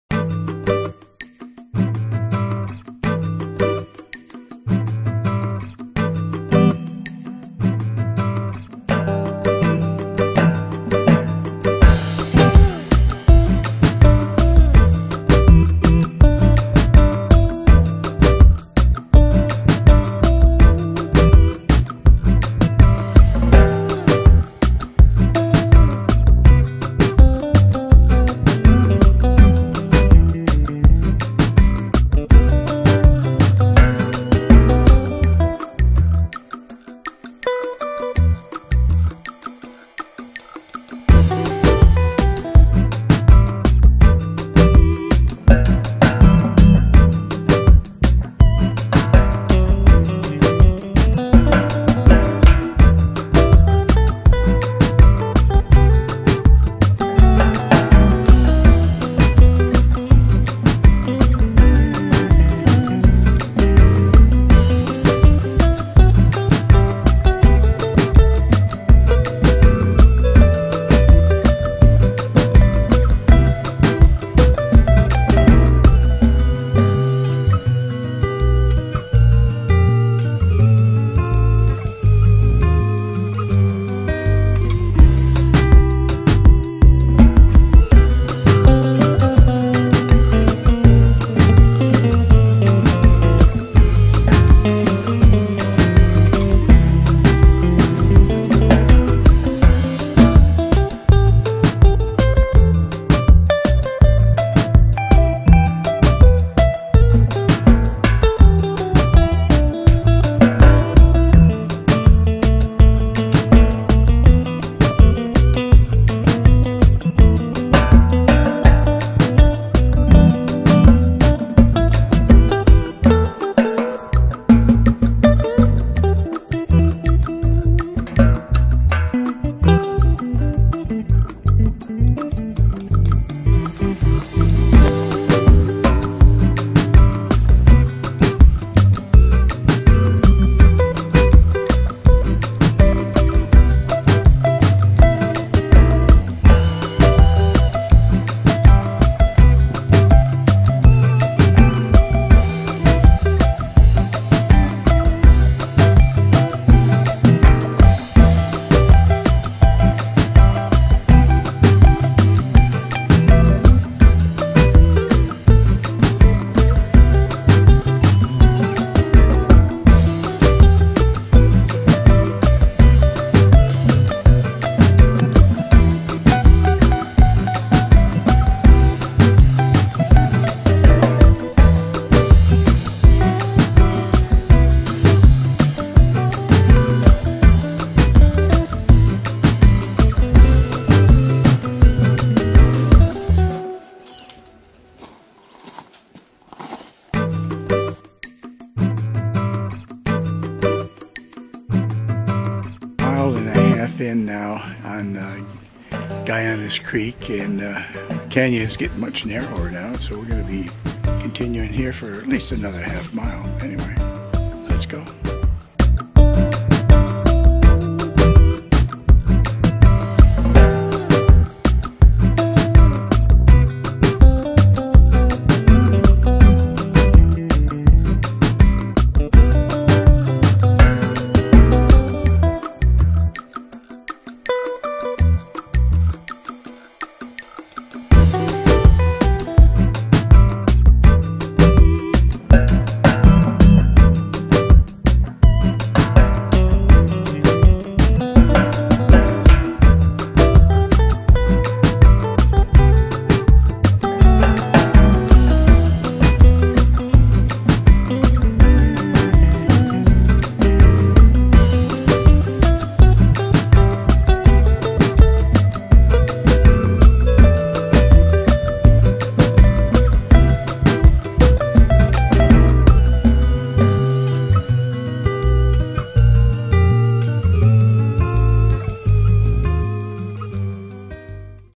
Lower Gallinas Trail # 795, New Mexico - April 19, 2022
The trailhead starts from the shoulder of NM Hwy 152 and immediately drops in elevation over the first 1/2 mile, dropping 280 ft. to the juncture with the Gallinas Canyon.  From this juncture the user path crosses the creek numerous times -- all of which are manageable during the dry season.  We proceeded 2 miles along the creek before returning.